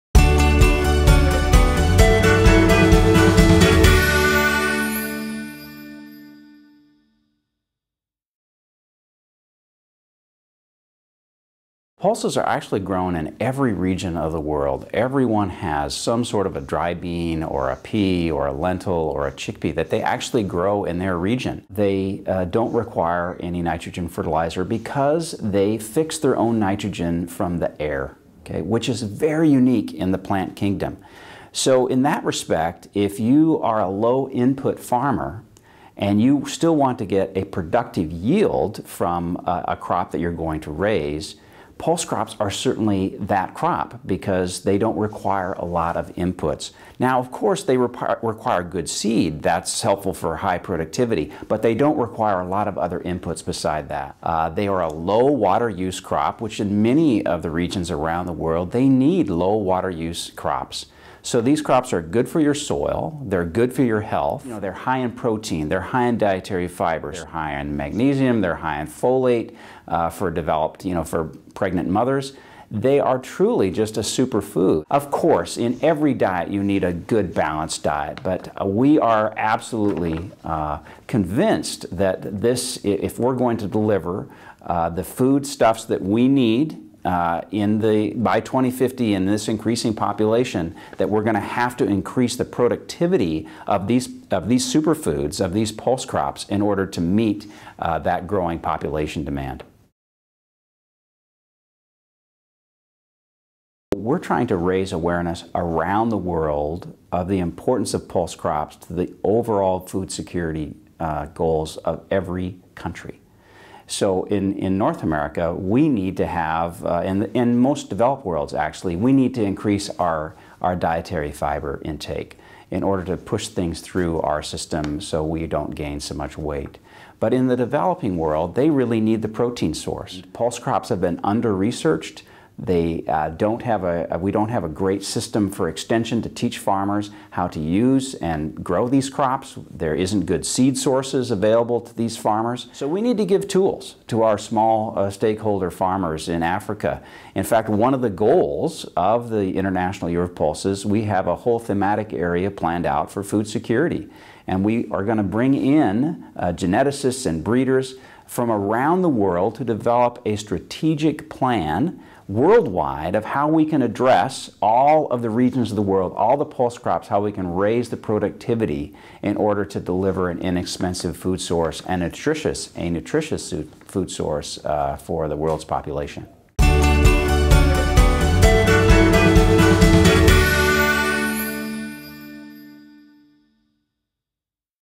Why Do We Need an International Year of Pulses?
The United Nations has declared that 2016 will be the International Year of Pulses. What lead to this decision? How can pulses help the health of our population and our planet? Tim McGreevy, CEO of the USA Dry Pea & Lentil Council tells Farming First TV.